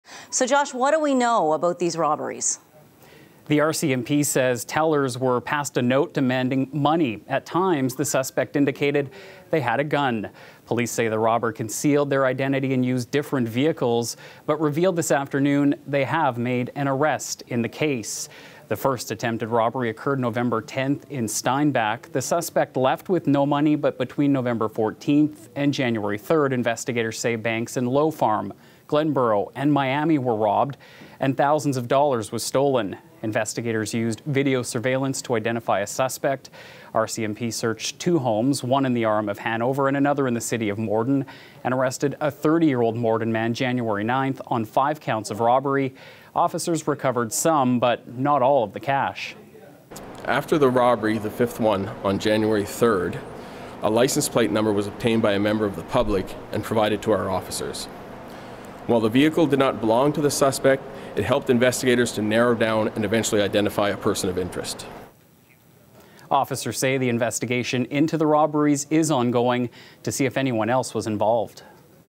Investigation continues: Man charged in string of small-town bank robberies in southern Manitoba
A 30-year-old man has been charged in connection with five bank robberies in less than two months across southern Manitoba.

The man, from Morden, was arrested Jan. 9 after different vehicles were used to rob banks in Steinbach (Nov. 10), Lowe Farm (Nov. 14), Glenboro (Dec. 8) and the same institution in Miami on two occasions (Dec. 23 and Jan. 3).

In all but the Steinbach robbery, the man fled with money after handing tellers a note suggesting he had a gun.

"We're hoping that today's announcement brings back a sense of safety to all the employees and communities that were affected," RCMP Insp. Tim Arseneault said.

Police said the man used different vehicles that did not belong to him. A witness in one case got a licence plate number and police used it to narrow the investigation, which led to the arrest on Jan. 9.